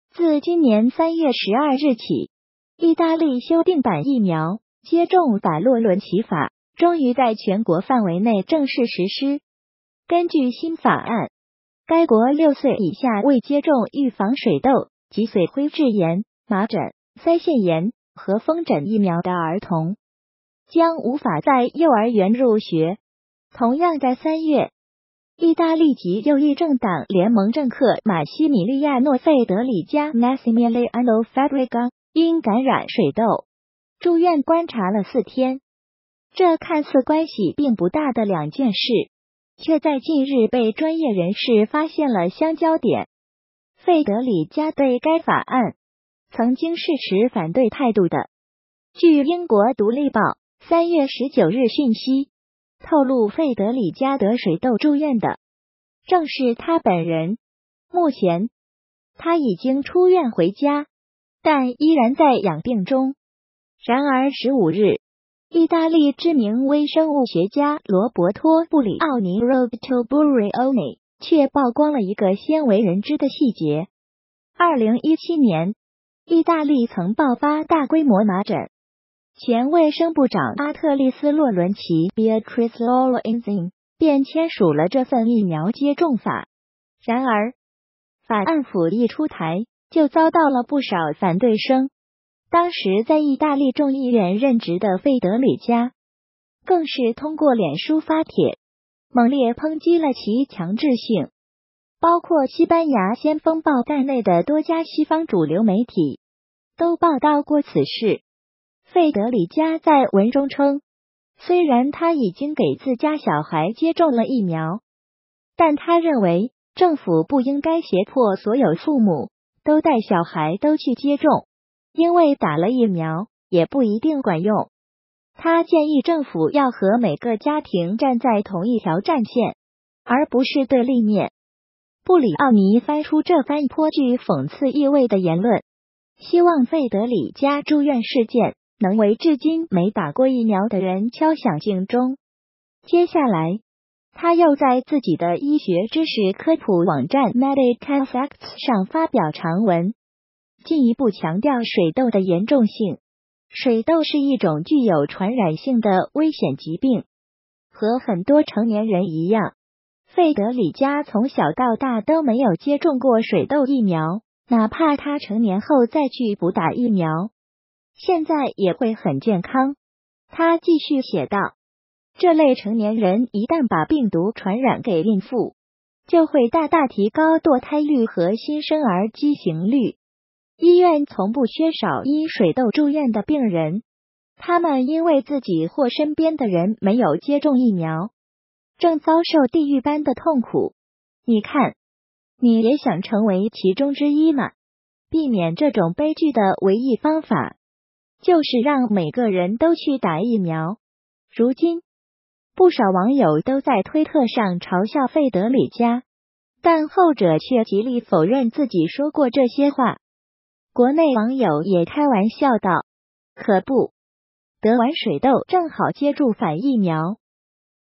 曾反對水痘疫苗的他 得水痘了…
自今年3月12日起，義大利修訂版疫苗接種法“洛倫齊法”終於在全國範圍內正式實施。這看似關係並不大的兩件事，卻在近日被專業人士發現了相交點：費德里加對該法案，曾經是持反對態度的…